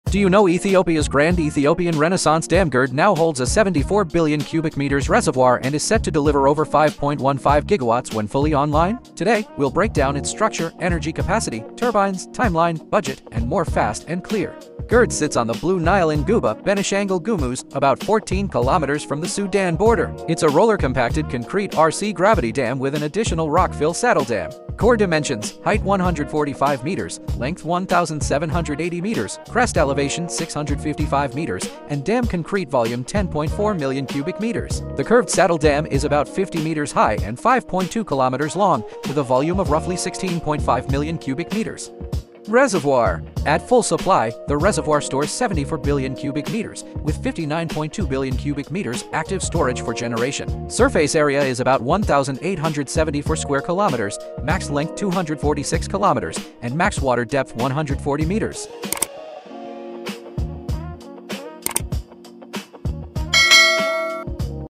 Inside GERD: Ethiopia’s Grand Renaissance Dam (Specs, Capacity & Timeline)PART-1 3#shorts
Explore Ethiopia’s Grand Ethiopian Renaissance Dam (GERD) on the Blue Nile! 🌊💡
Discover its structure, reservoir, turbines, energy capacity, timeline, and construction cost—all in one clear breakdown. From the RCC gravity dam and rock-fill saddle dam to Africa’s largest hydropower plant with 5.15 GW capacity, get the full picture of this engineering marvel.

📌 Key Facts:

Height / Length: 145 m / 1,780 m

Reservoir: 74 km³ (Active: 59.2 km³)

Turbines: 13 Francis (11×400 MW, 2×375 MW)

Annual Generation: ~15.76 TWh

Cost: ~$5B, funded via government bonds & public contributions

Status (2025): ~98.66% complete, inauguration set for Sept 2025